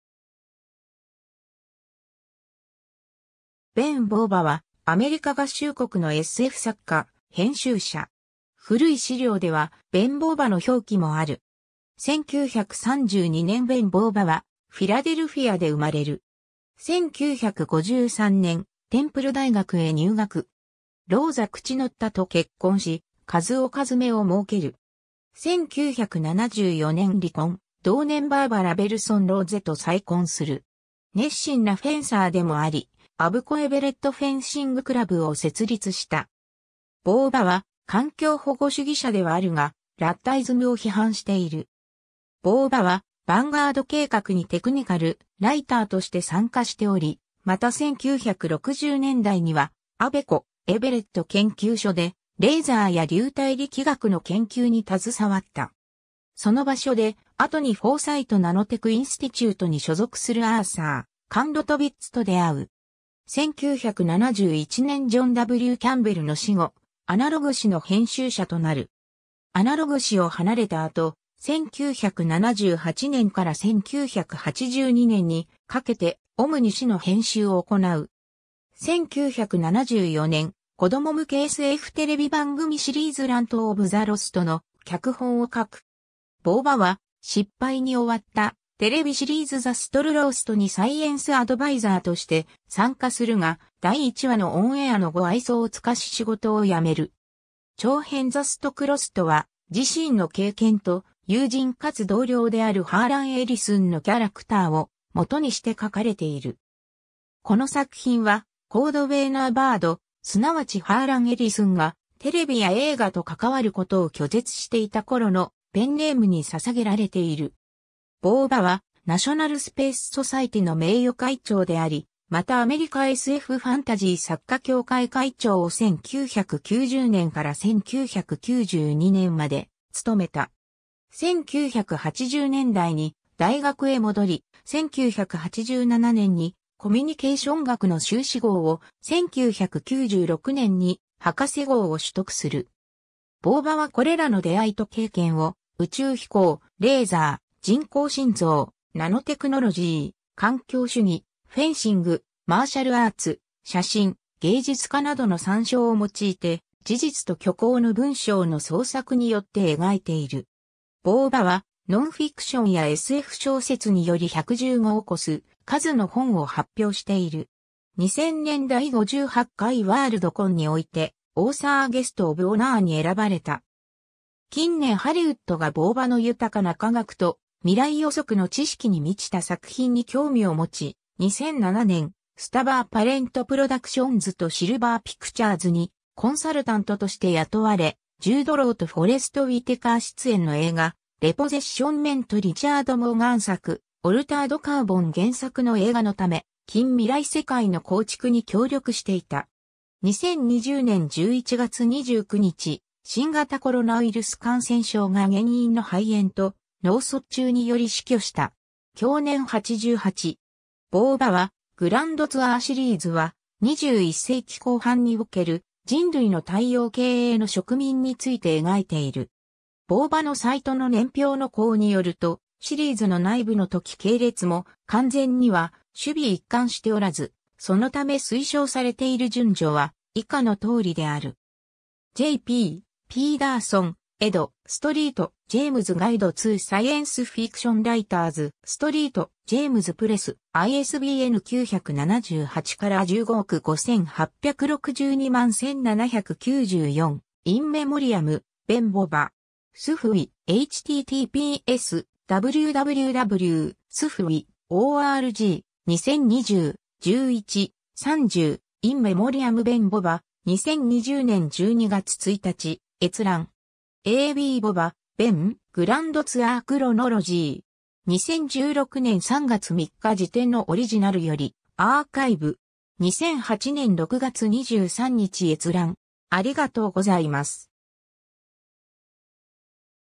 ベン・ボーヴァ
ベン・ボーヴァ（Ben Bova、1932年11月8日 - 2020年11月29日）は、アメリカ合衆国のSF作家、編集者。
 古い資料ではベン・ボバの表記もある。
 1932年ベン・ボーヴァはフィラデルフィアで生まれる。
1953年テンプル大学へ入学。
ローザ・クチノッタと結婚し、一男一女をもうける。
1974年離婚、同年バーバラ・ベルソン・ローゼと再婚する。
 熱心なフェンサーでもあり、アブコ・エベレット・フェンシング・クラブを設立した。
ボーヴァは環境保護主義者ではあるが、ラッダイズムを批判している。
 ボーヴァはヴァンガード計画にテクニカル・ライターとして参加しており、また1960年代にはAVCO-Everett研究所でレーザーや流体力学の研究に携わった。
その場所で、後にフォーサイト・ナノテク・インスティテュートに所属するアーサー・カンロトヴィッツと出会う。
 1971年ジョン・W・キャンベルの死後アナログ誌の編集者となる。
アナログ誌を離れた後、1978年から1982年にかけて『オムニ』誌の編集を行う。
 1974年子供向けSFテレビ番組シリーズ Land of the Lost の脚本を書く。
 ボーヴァは失敗に終わったテレビシリーズ The Starlost にサイエンス・アドバイザーとして参加するが、第1話のオンエアの後愛想を尽かし仕事を辞める。
長編 The Starcrossed (1975) は自身の経験と、友人かつ同僚であるハーラン・エリスンのキャラクターを元にして書かれている。
この作品は「コードウェイナー・バード」、即ちハーラン・エリスンがテレビや映画と関わることを拒絶していた頃のペンネームに捧げられている。
 ボーヴァはナショナル・スペース・ソサエティの名誉会長であり、またアメリカSFファンタジー作家協会会長を1990年から1992年まで務めた。
 1980年代に大学へ戻り、1987年にコミュニケーション学の修士号を、1996年に博士号を取得する。
 ボーヴァはこれらの出会いと経験を、宇宙飛行、レーザー、人工心臓、ナノテクノロジー、環境主義、フェンシング、マーシャル・アーツ、写真、芸術家などの参照を用いて、事実と虚構の文章の創作によって描いている。
 ボーヴァはノンフィクションやSF小説により115を越す数の本を発表している。
2000年第58回ワールドコンにおいて、オーサー・ゲスト・オブ・オナーに選ばれた。
 近年ハリウッドがボーヴァの豊かな科学と未来予測の知識に満ちた作品に興味を持ち、2007年スタバー・パレント・プロダクションズとシルバー・ピクチャーズにコンサルタントとして雇われ、ジュード・ロウとフォレスト・ウィテカー出演の映画『レポゼッション・メン』（Repossession Mambo）とリチャード・モーガン作『オルタード・カーボン』原作の映画のため、近未来世界の構築に協力していた。
 2020年11月29日、新型コロナウイルス感染症（COVID-19）が原因の肺炎と脳卒中により死去した。
享年88。
 ボーヴァは Grand Tour シリーズは、21世紀後半における人類の太陽系への植民について描いている。
ボーヴァのサイトの年表の項によると、 シリーズの内部の時系列も完全には首尾一貫しておらず、そのため推奨されている順序は以下の通りである。
^ Jay P. Pederson, ed (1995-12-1). St. James Guide to Science Fiction Writers (4th edition ed.). St. James Press. ISBN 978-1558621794 ^ “In Memoriam: Ben Bova” (英語). SFWA. (2020年11月30日). 2020年12月1日閲覧。
 ^ a b Bova, Ben. “Grand Tour Chronology”. 2016年3月3日時点のオリジナルよりアーカイブ。
2008年6月23日閲覧。